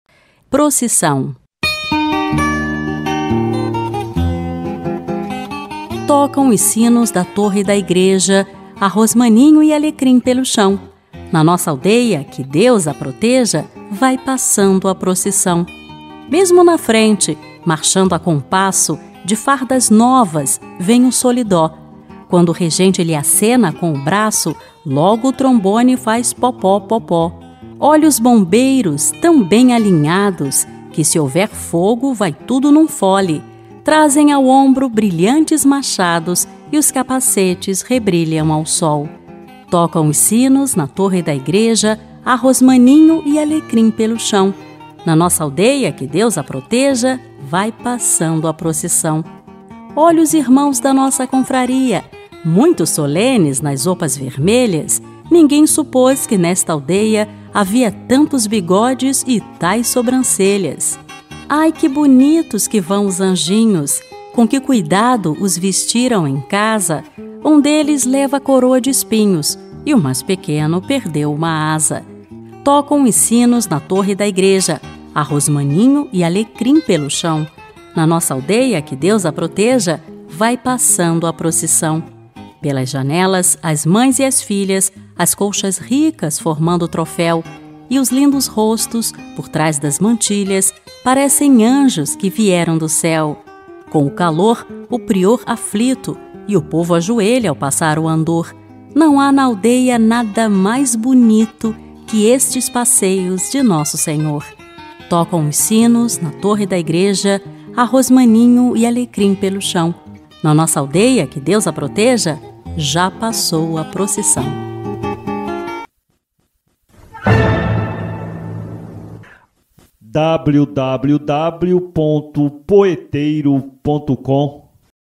Procissão (Poema), de Antônio Lopes Ribeiro
PROCISSÃO

Tocam os sinos da torre da igreja,
Há rosmaninho e alecrim pelo chão.
Na nossa aldeia que Deus a proteja!
Vai passando a procissão.

Mesmo na frente, marchando a compasso,
De fardas novas, vem o solidó.
Quando o regente lhe acena com o braço,
Logo o trombone faz popó, popó.

Olha os bombeiros, tão bem alinhados!
Que se houver fogo vai tudo num fole.
Trazem ao ombro brilhantes machados,
E os capacetes rebrilham ao sol.

Tocam os sinos na torre da igreja,

Olha os irmãos da nossa confraria!
Muito solenes nas opas vermelhas!
Ninguém supôs que nesta aldeia havia
Tantos bigodes e tais sobrancelhas!

Ai, que bonitos que vão os anjinhos!
Com que cuidado os vestiram em casa!
Um deles leva a coroa de espinhos.
E o mais pequeno perdeu uma asa!

Pelas janelas, as mães e as filhas,
As colchas ricas, formando troféu.
E os lindos rostos, por trás das mantilhas,
Parecem anjos que vieram do Céu!

Com o calor, o Prior aflito.
E o povo ajoelha ao passar o andor.
Não há na aldeia nada mais bonito
Que estes passeios de Nosso Senhor!

Já passou a procissão.